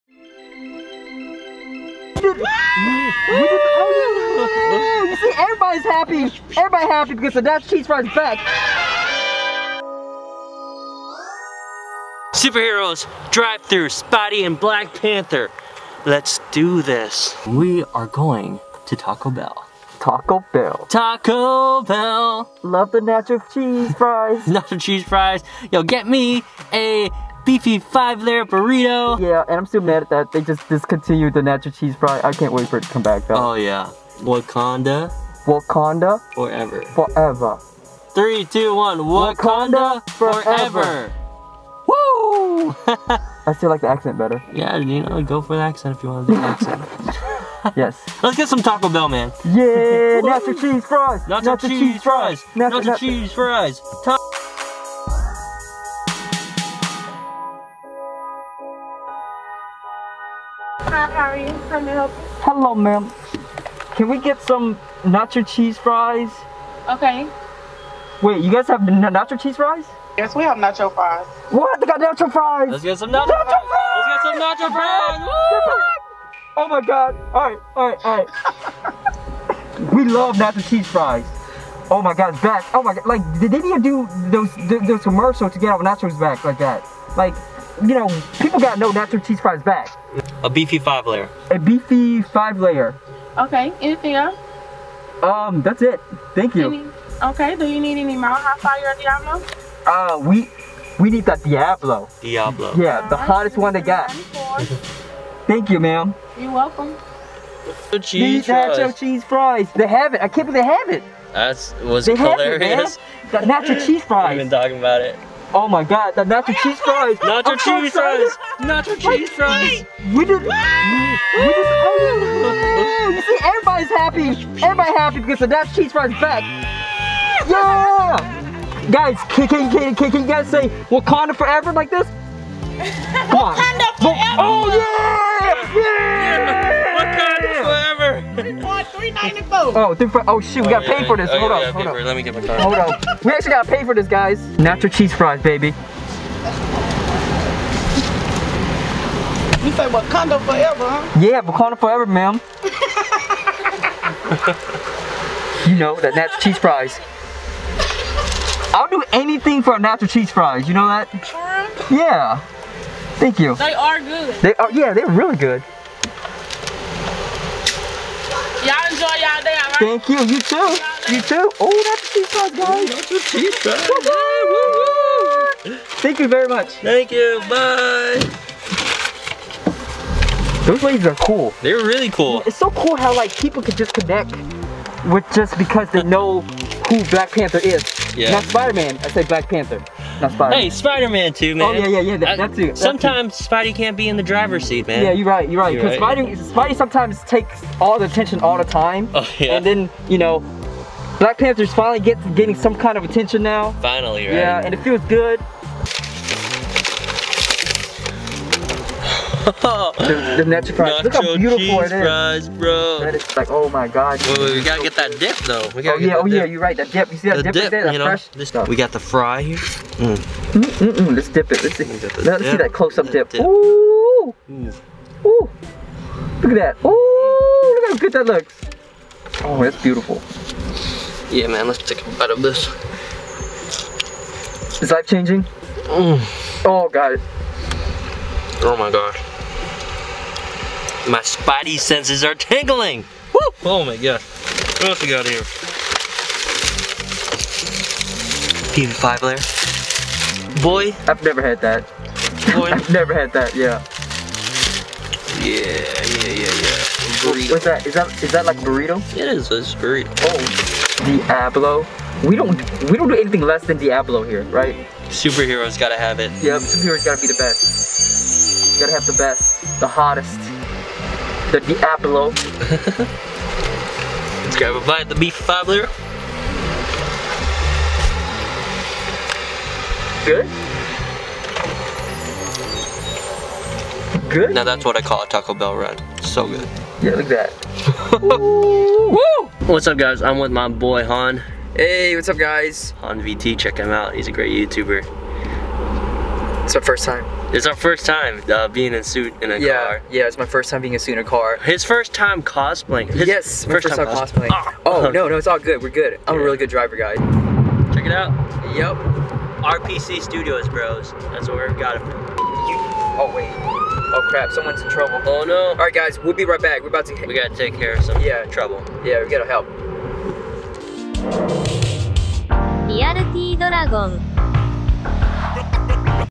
BLACK PANTHER & SPIDER-MAN IN REAL LIFE - DRIVE THRU FAST FOOD - RealTDragon
BLACK PANTHER & SPIDER-MAN DRIVE THRU IN REAL LIFE PART 4

Spider-Man cosplayer - @RealTDragon